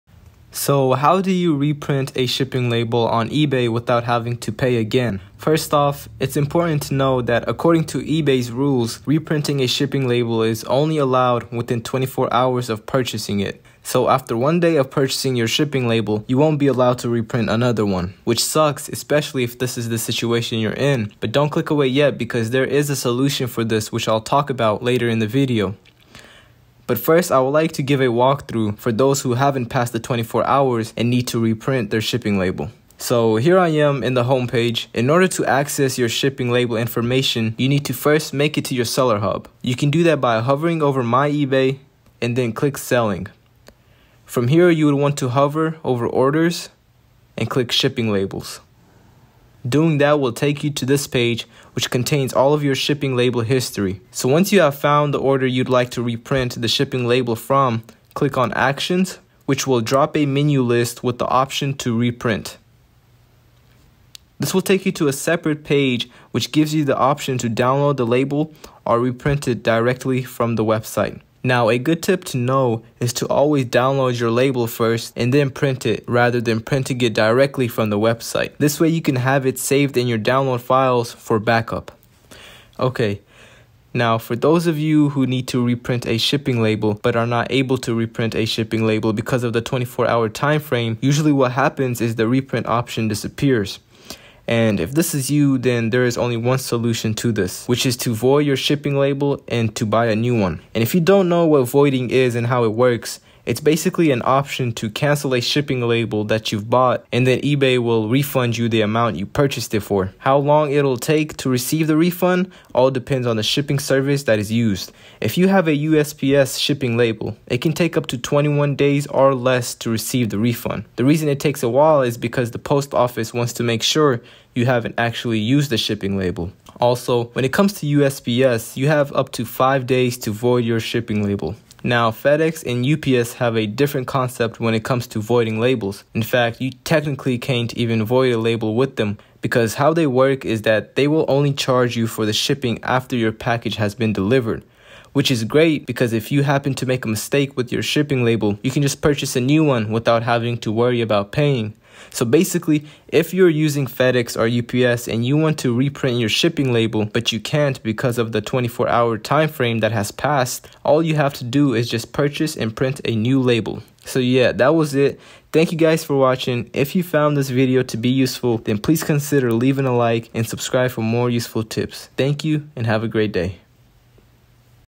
EBAY | How To Reprint A Shipping Label Without Having To Pay Again
In this video I'll be giving a walkthrough on how you can reprint a shipping label without having to pay again. I also talk about how you can reprint a label if the reprint option disappears.